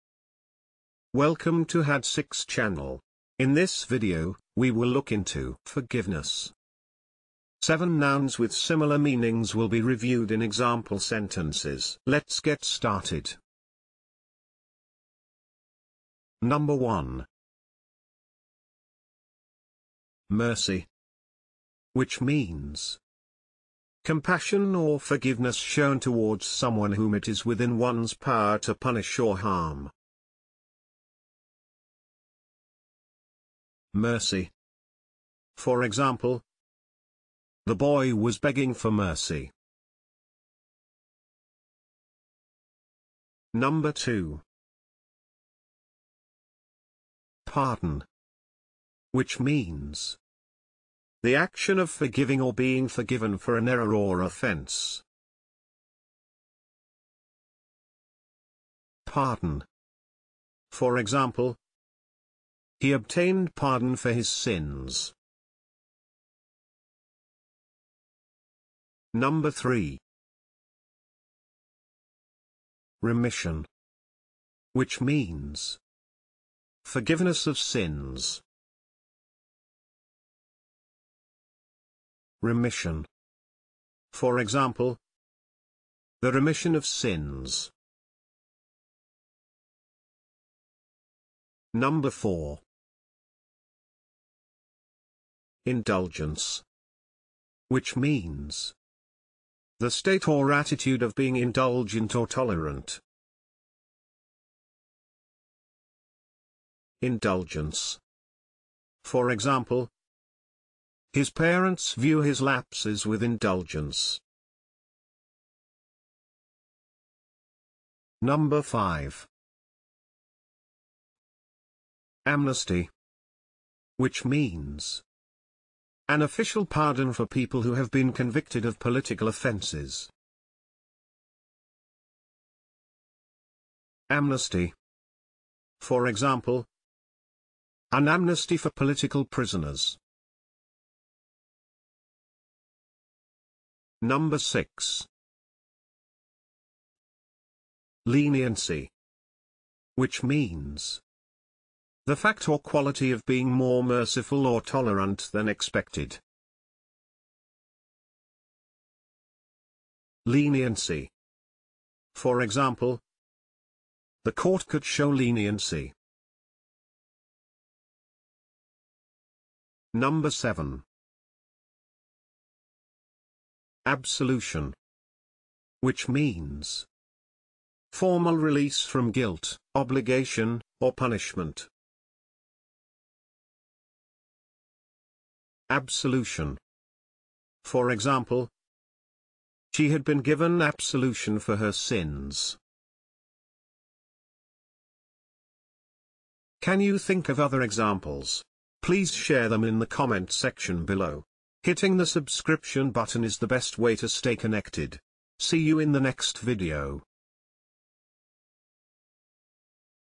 forgiveness - 7 nouns which mean forgiveness (sentence examples)
This video examines forgiveness as a noun. Here, 7 nouns, which have meanings similar to that of forgiveness in standard English, are introduced through sentence examples. These synonyms help you to remember the meanings of this group of words for improving your vocabulary.